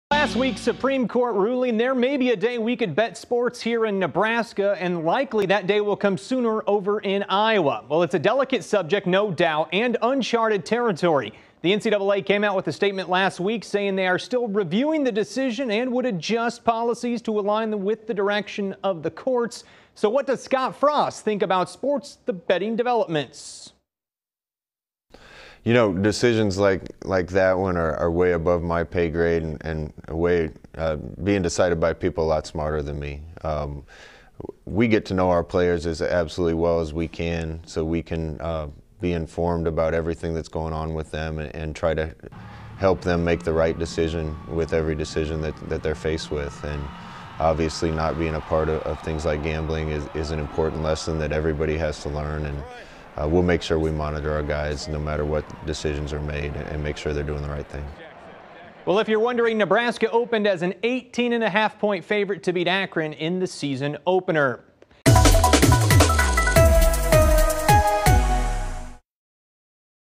Scott Frost weighs in legalized gambling
Matt Lothrop reports.